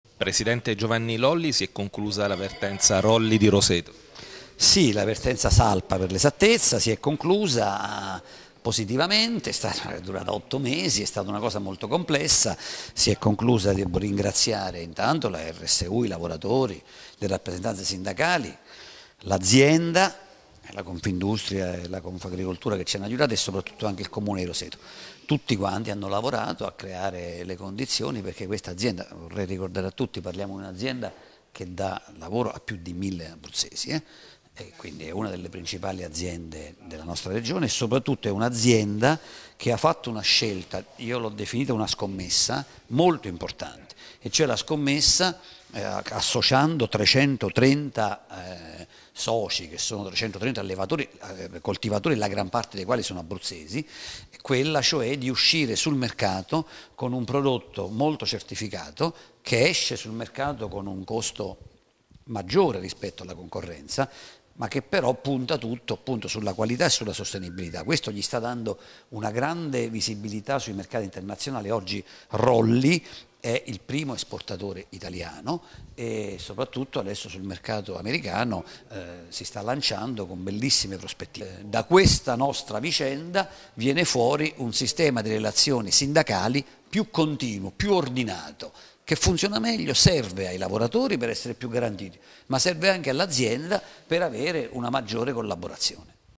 Chiusura positiva per la vertenza Salpa di Roseto
Il presidente vicario della Regione Abruzzo, Giovanni Lolli, che ha anche la delega allo sviluppo economico, ha annunciato la chiusura positiva della vertenza Rolli Salpa di Roseto degli Abruzzi. Ascoltiamo l'intervista